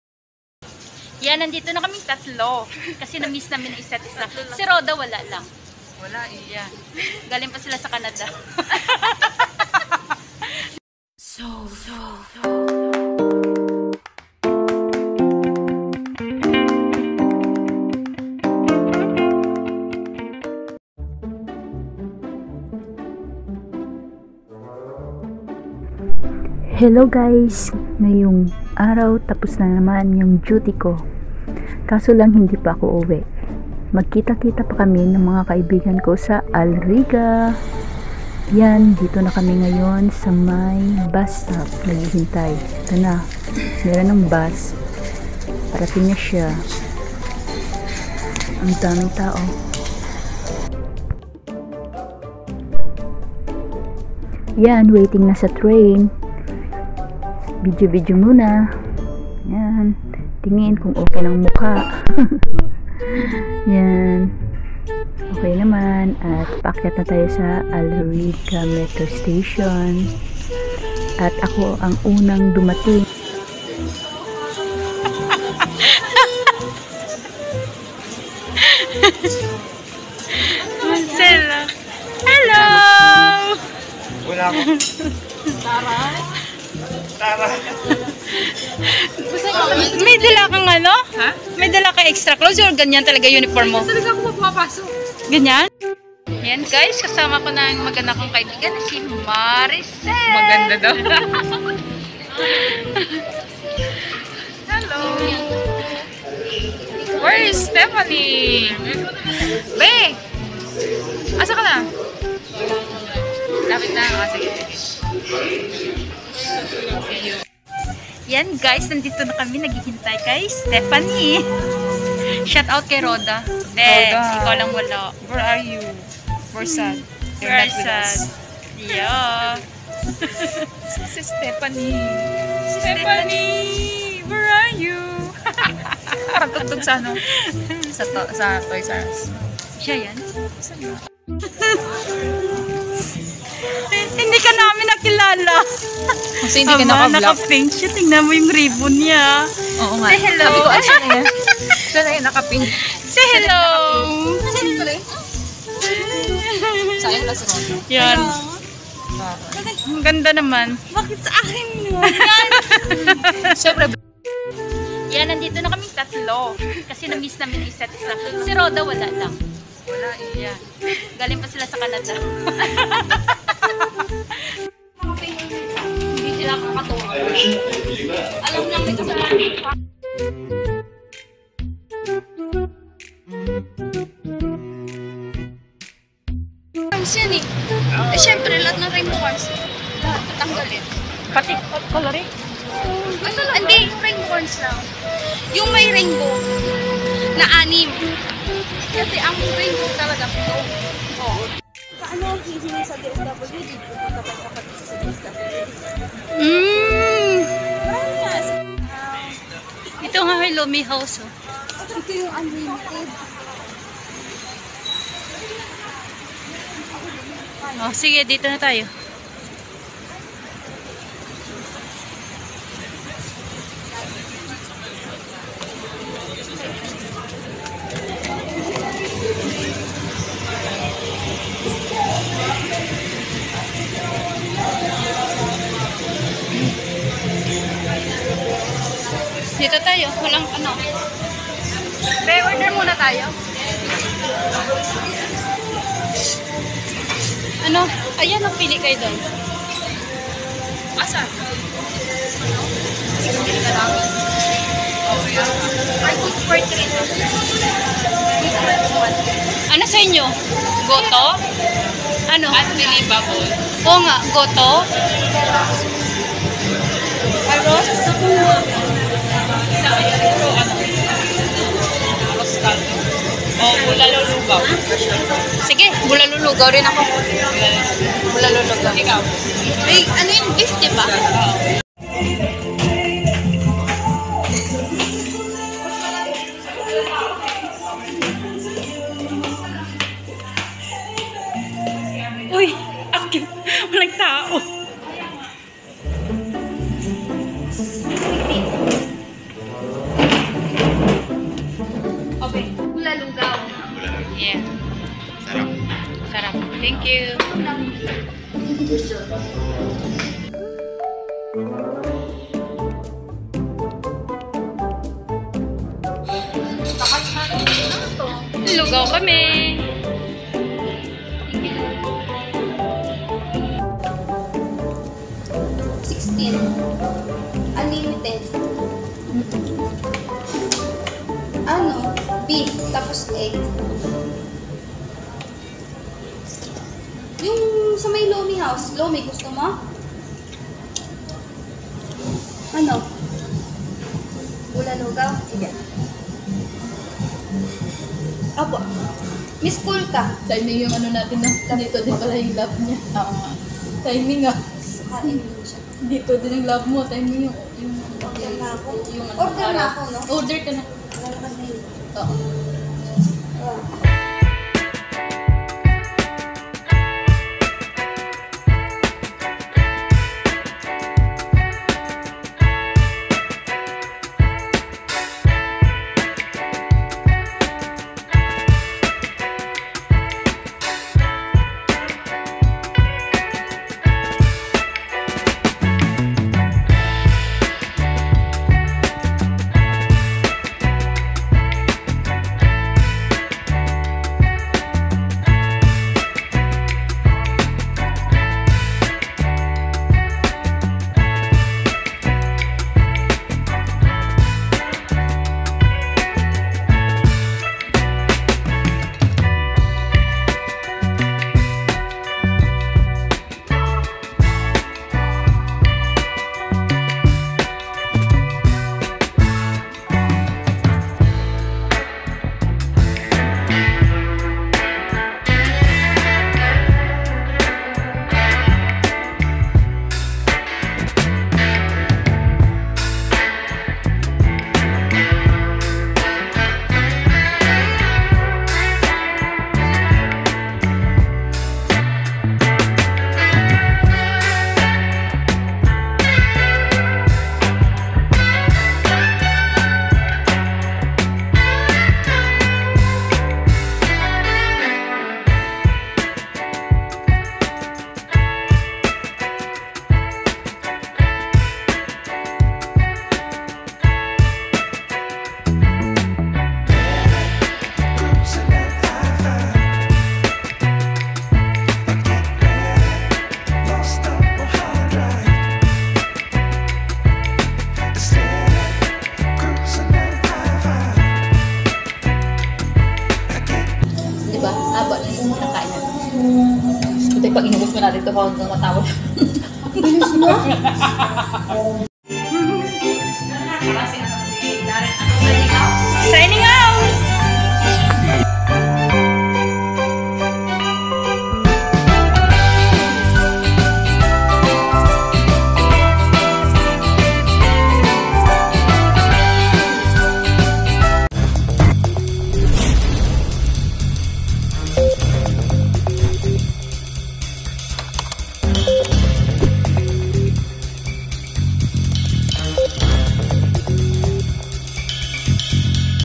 Miss na namin ang ISAT ISA kaya nag LOMI HOUSE kami / BONDING TIME
Miss na namin ang ISAT  ISA  kaya nag LUMI HOUSE  kami / BONDING TIME